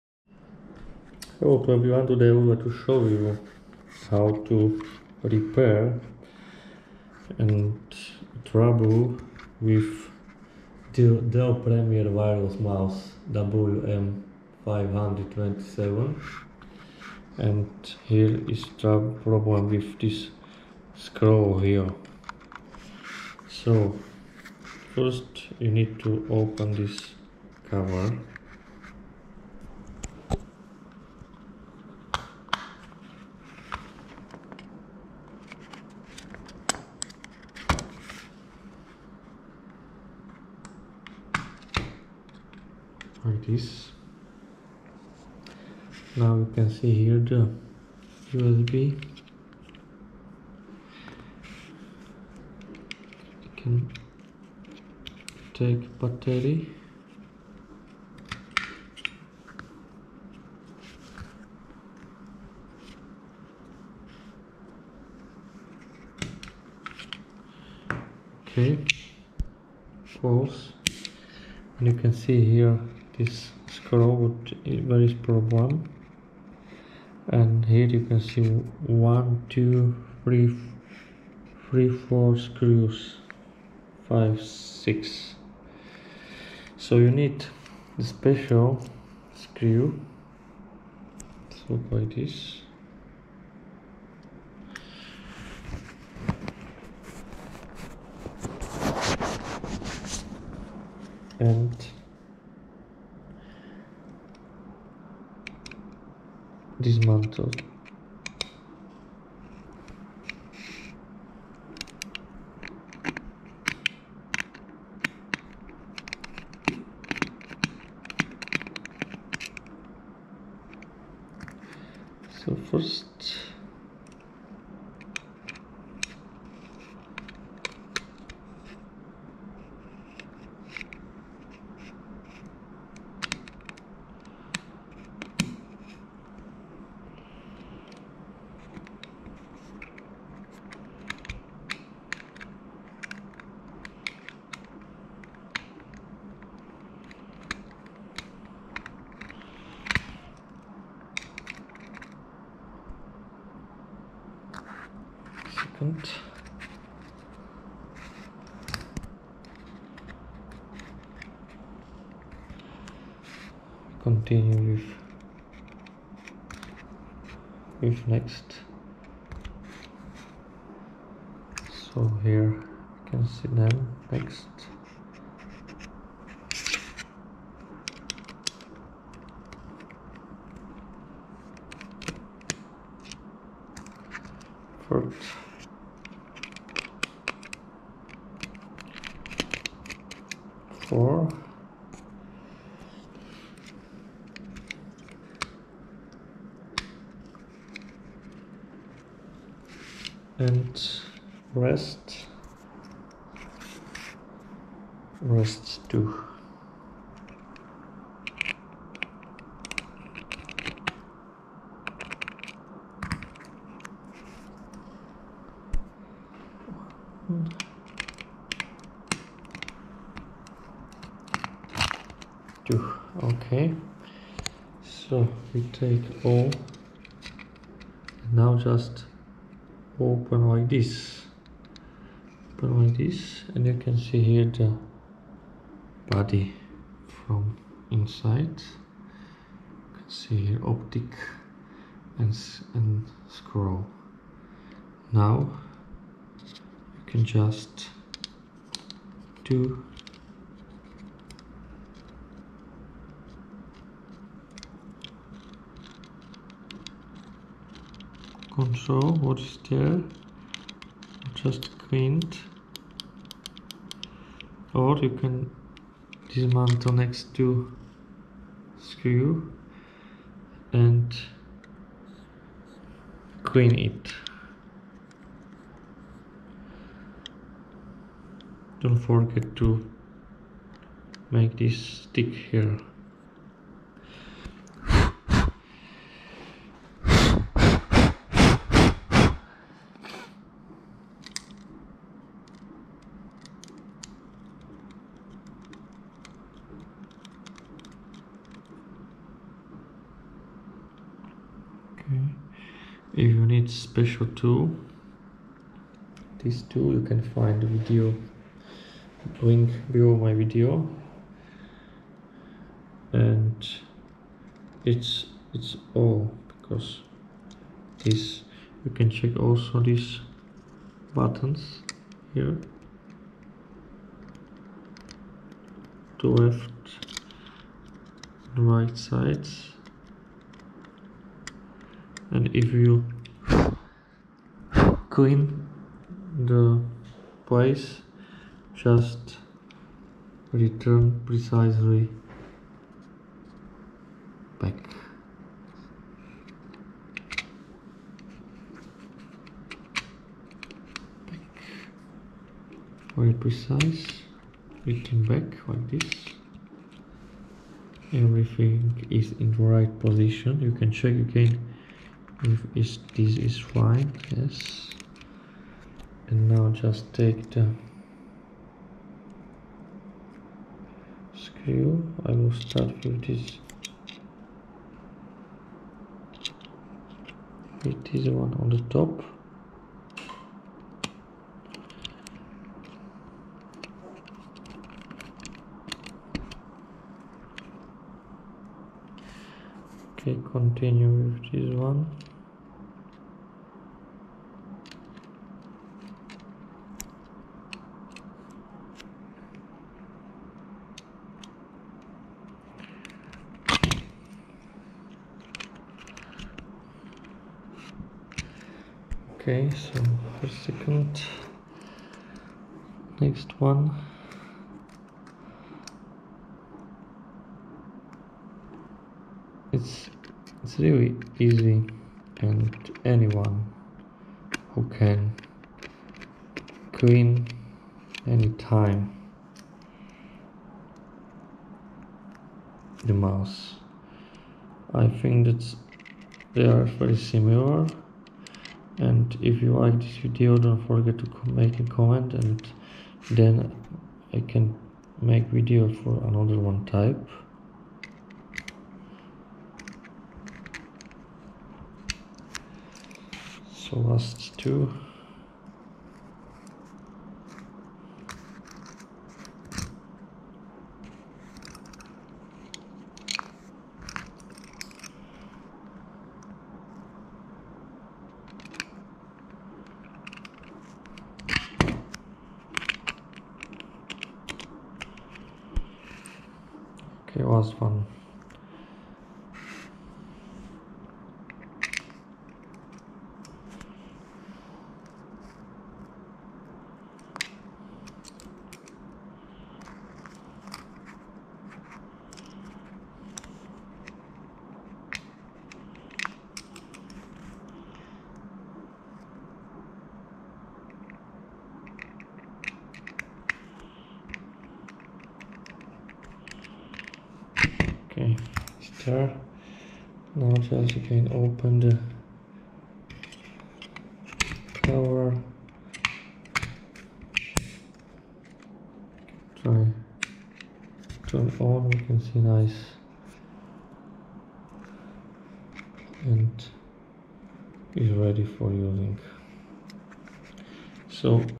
How to Troubleshoot MOUSE DELL WM527
When you have a problem with scrolling wheel sometimes need just dismantle and little clean, maybe mechancal dirty..

Dell KM717 Premier Wireless Keyboard and Mouse BEST PRICE